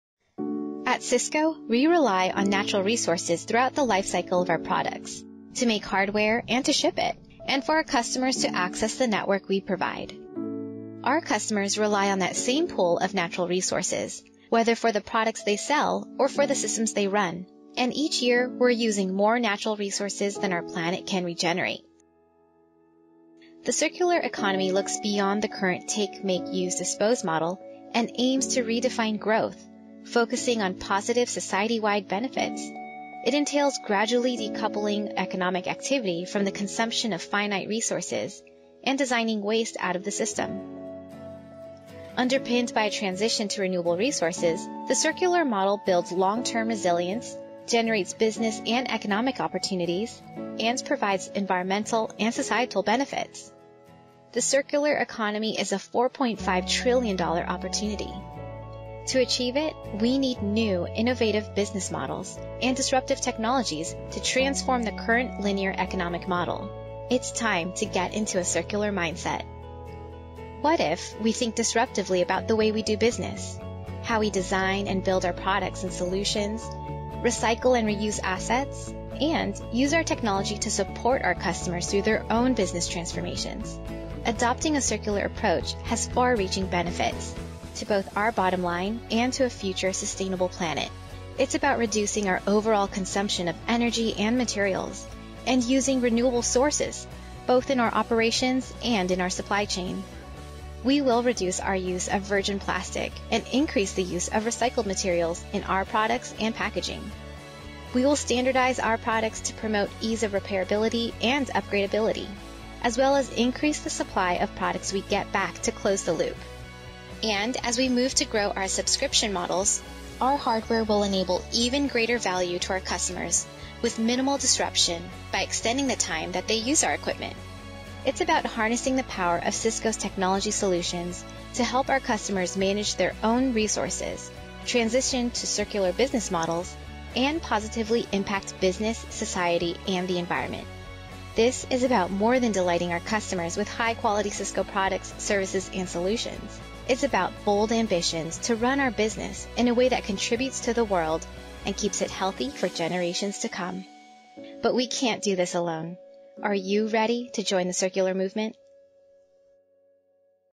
シスコの循環経済への取り組み
シスコは循環経済の加速に注力しています。製品とソリューションの設計から開発、提供の方法を変革し、リサイクルやリユースを推進します。